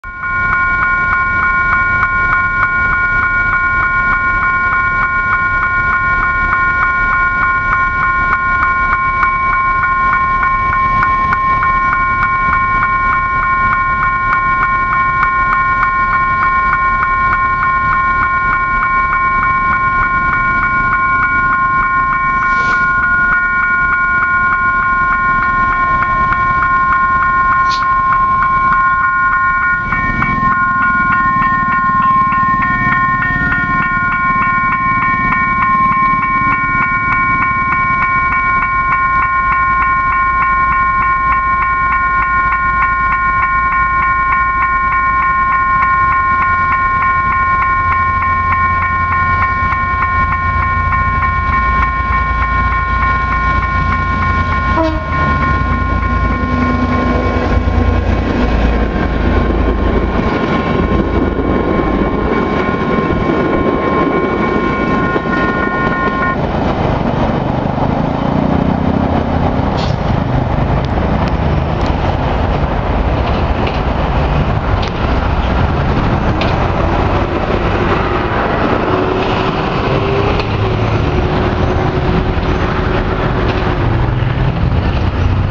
BARRERAS DE UNLaM en San Justo HD-TY 100_7200.AVI
No me acuerdo la calle, jaja x eso le puse "DE LA U.N.La M." x la universidad cercana.
vemos las barreras, saxby-FM, con un autotrol. barreras de 5m. loc GM GR12w A630 x San Justo, hacia Haedo.